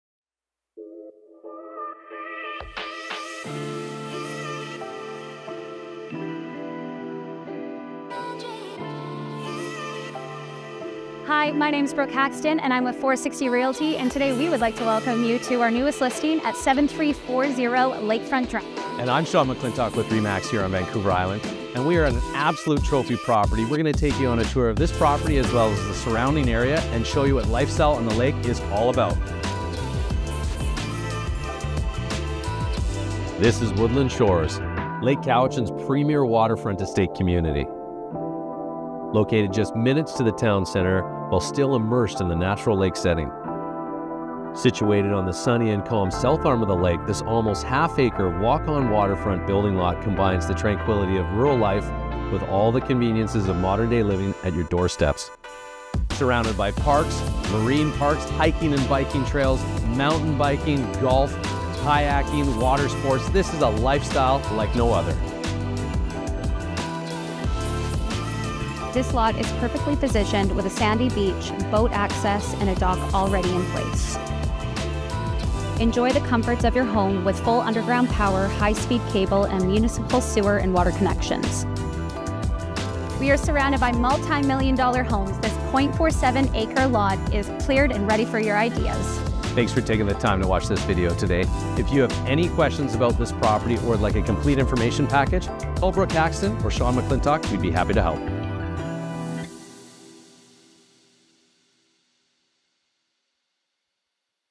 7340 Lakefront drive, Lake Cowichan
Woodland Shores - Lake Cowichan's premiere waterfront community.Mins to town centre & immersed in natrl lake setting by mtn & lake views.Situated on sunny & calm south arm.Almost 1/2 acre walk-on-waterfront lot combines tranquility of rural life w/ conveniences of modern living at your door steps.Small rock/sandy beach w/ gentle slope & gradual lake access make this ideal spot for kids/older people alike.The 85'x 240' lot dimensions provide 7000 sqft build envelope, ideally suited for main lvl entry w/or w/out add'l accom. Cleared, ready to construct dream home.Lot includes mature fir & arbutus trees giving established feel providing privacy from neighbours & shade in summertime.Already built dock w/ loads of room for all the toys while you construct new home.Surrounded by parks, hiking & biking trails, water sports, & all levels of world class schools. Other features; brand new booms, underground power/high speed cable w/ municipal sewer/water.(Info should be verified if important)